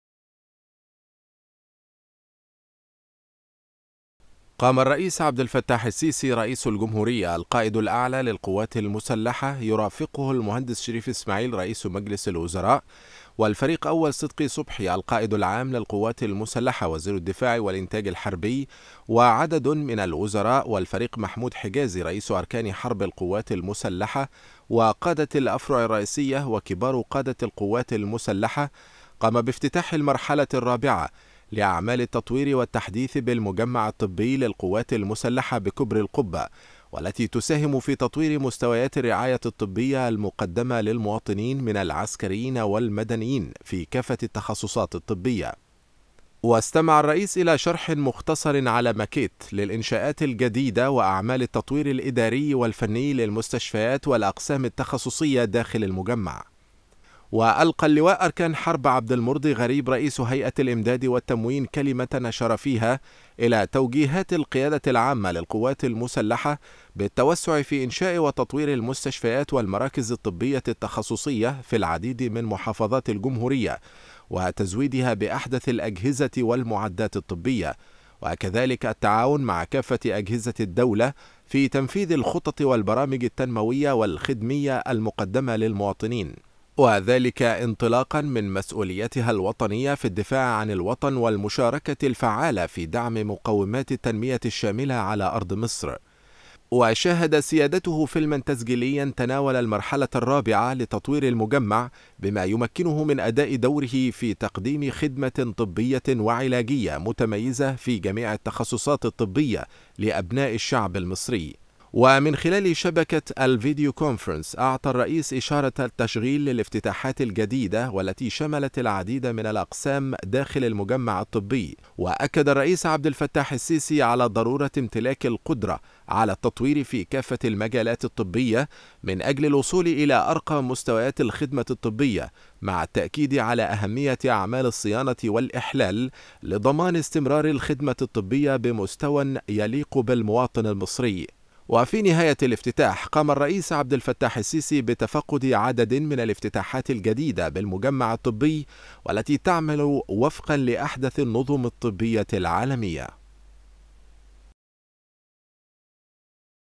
الرئيس عبد الفتاح السيسى يفتتح المرحلة الرابعة لتطوير المجمع الطبى بكوبرى القبة
افتتح السيد الرئيس عبد الفتاح السيسى رئيس الجمهورية القائد الأعلى للقوات المسلحة المرحلة الرابعة لأعمال التطوير والتحديث بالمجمع الطبى للقوات المسلحة بكوبرى القبة، والتى تضم الأسبقية الثانية لإنشاء مستشفى القلب التخصصى، وإنشاء مستشفى الاستقبال والطوارئ والحوادث، ورفع كفاءة وتطوير مستشفى الباطنة، ومبنى العيادات الخارجية والمراكز التخصصية وعدد من المنشآت والمبانى الإدارية والخدمية بالمجمع